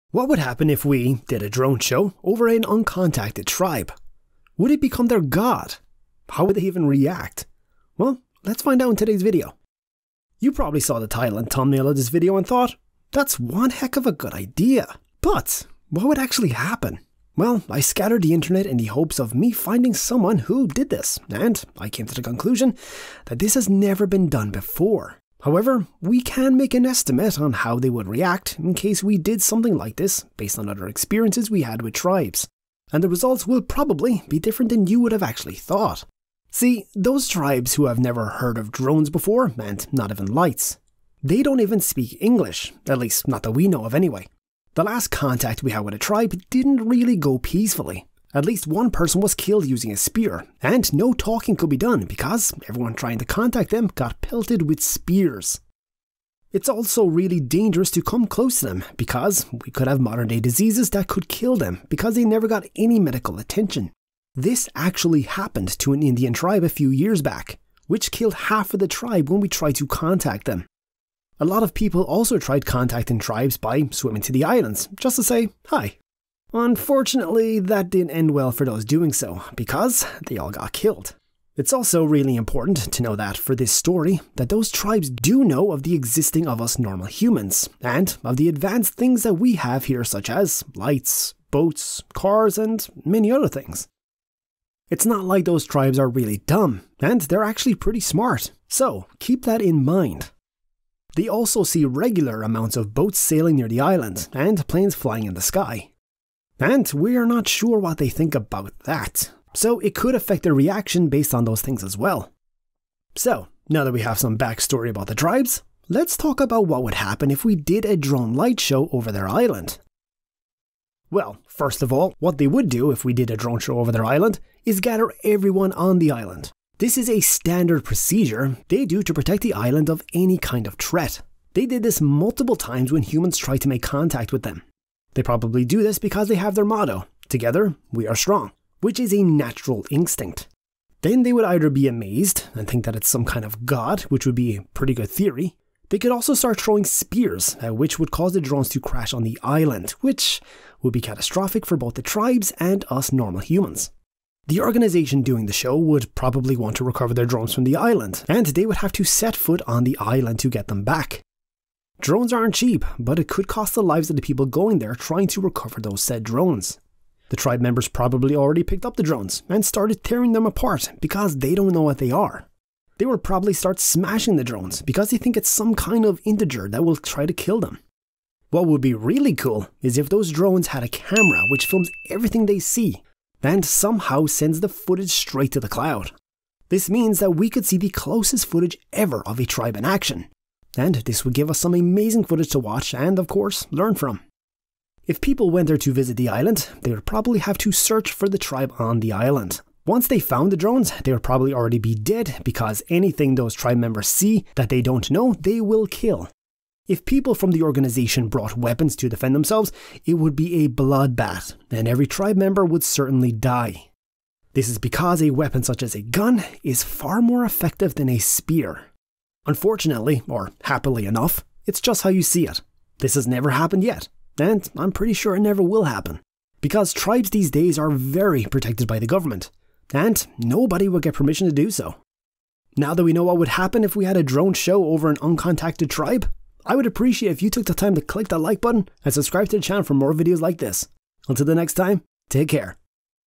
What If We Did A Drone Show Over An Uncontacted Tribe?
This recently went viral on Twitter with people questioning what would happen if we did a drone show over an uncontacted tribe. In this video we found out what would actually happen if we did decide to do a drone show over an uncontacted tribe.